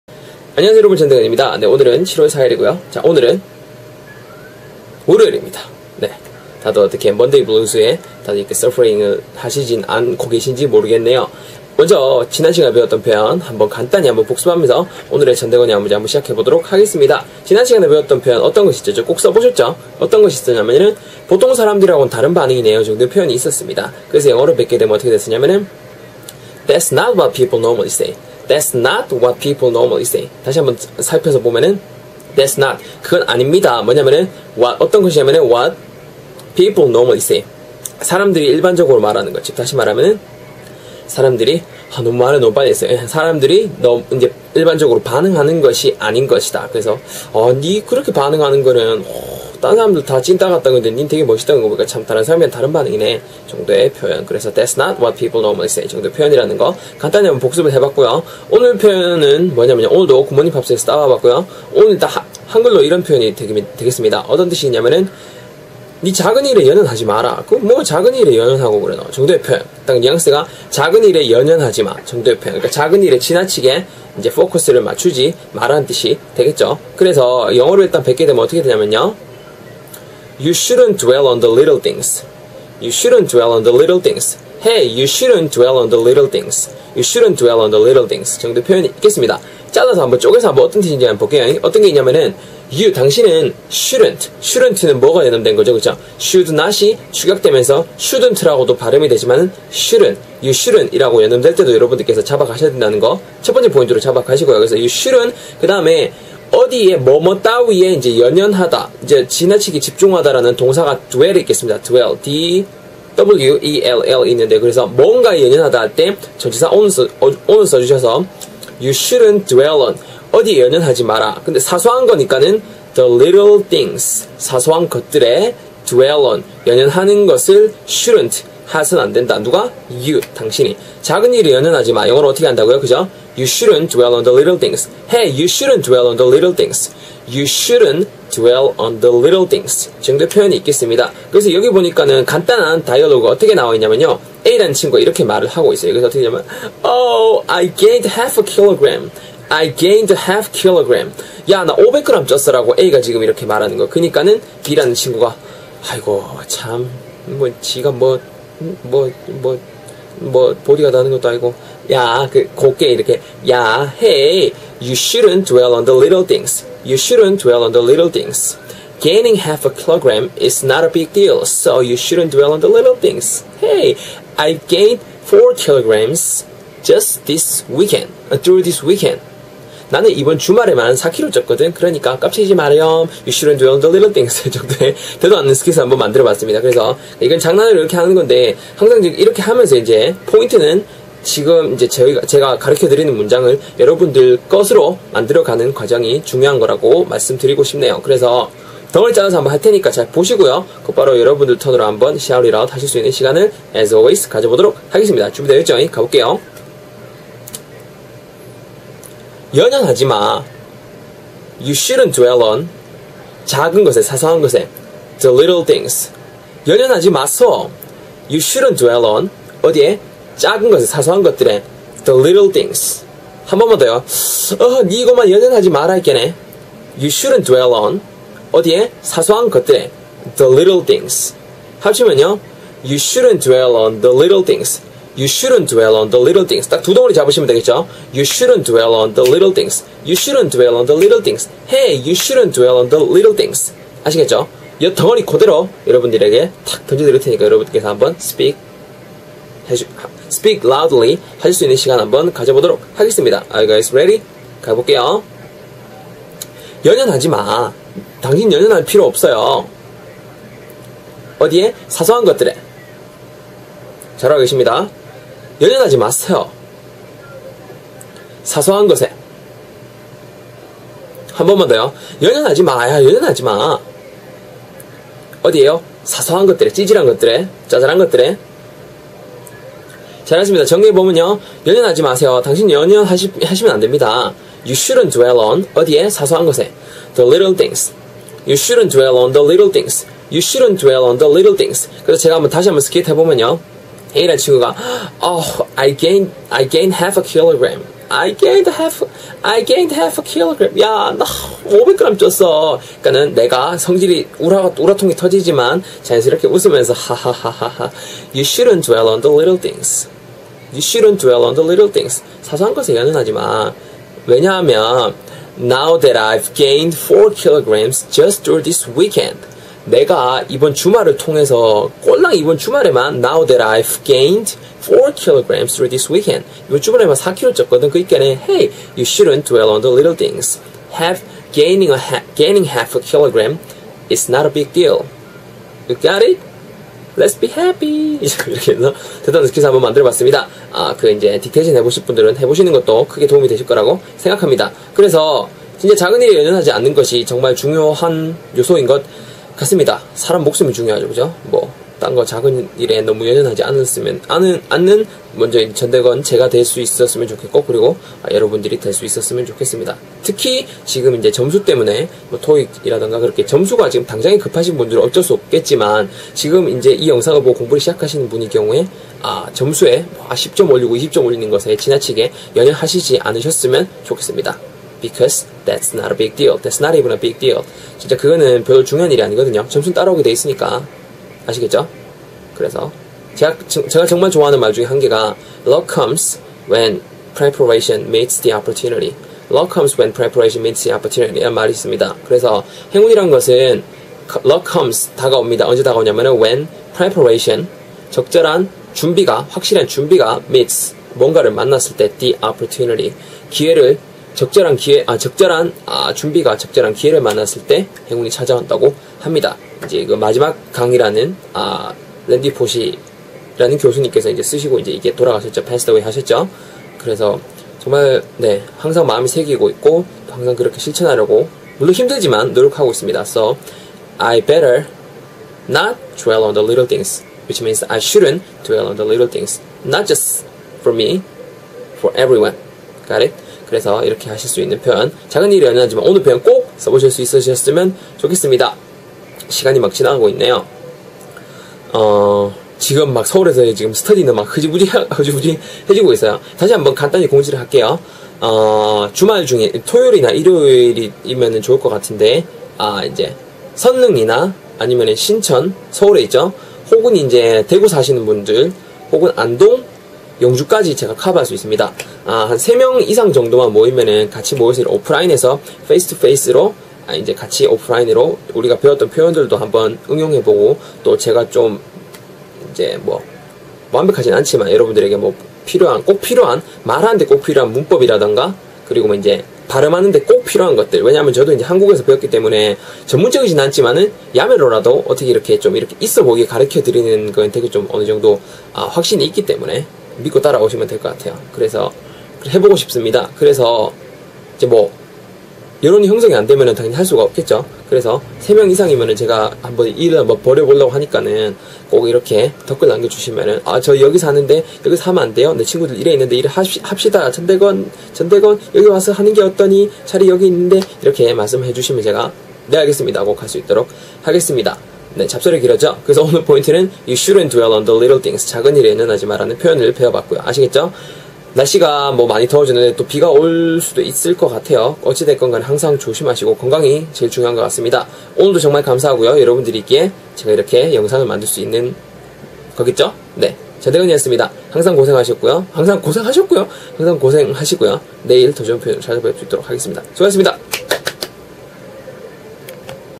'작은 일에 연연하지 마' 영어로는?
작은 일에 연연하지 마
You shouldn't dwell on the little things.